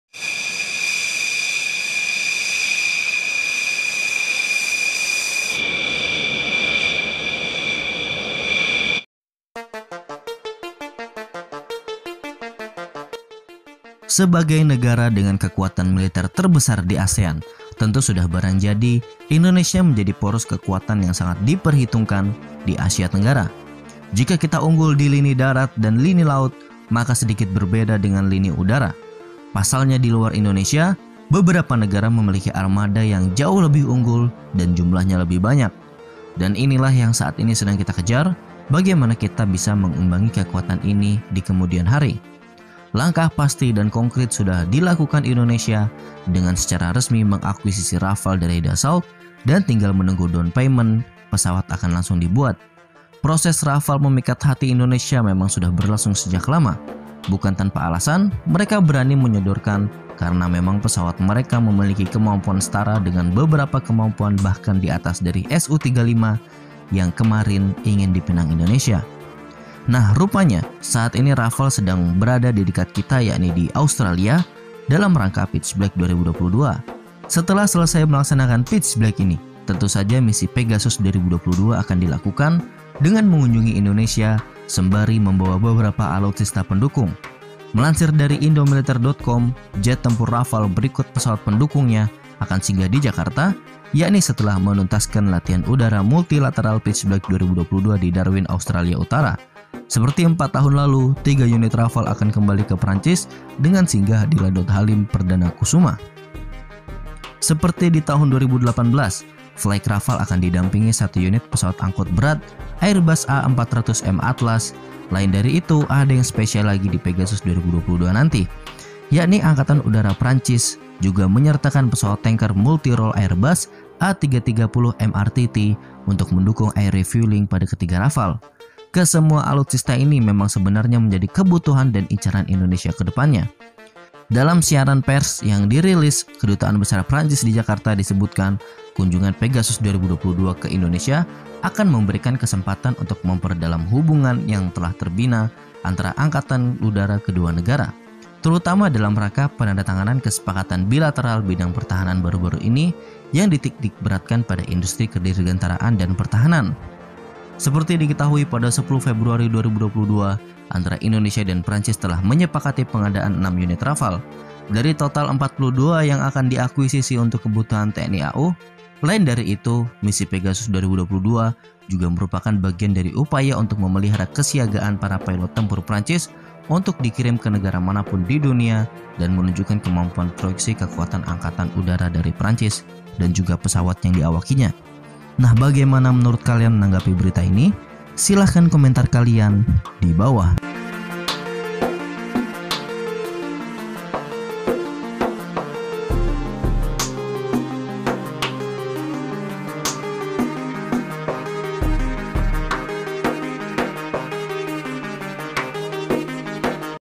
SIAPKAN UANG, RAFALE DATANGI INDONESIA SAMBIL BAWA A300 MRTT , GAMBARAN POWER TNI-AU MASA DEPAN
Semua berita kami sebutkan sumbernya didalam narasi

Sebagai negara dengan kekuatan militer terbesar di ASEAN tentu sudah barang jadi indonesia menjadi poros kekuatan yang sangat diperhitungkan di Asia tenggara. Jika kita unggul di lini darat dan lini laut maka sedikit berbeda dengan lini udara, pasalnya di luar Indonesia beberapa negara memiliki armada yang jauh lebih unggul dan jumlahnya lebih banyak, dan ini lah yang saat ini sedang kita kejar. Bagaimana kita bisa mengimbangi kekuatan ini dikemudian hari.